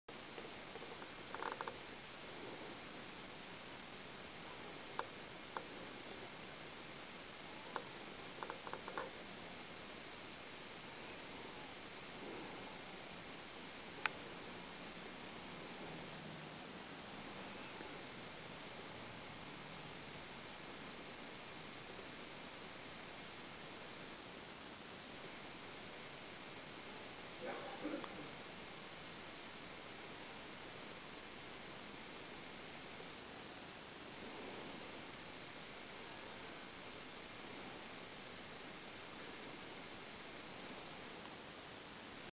Sun through the trees
Mom and Dads bedroom wall, midday...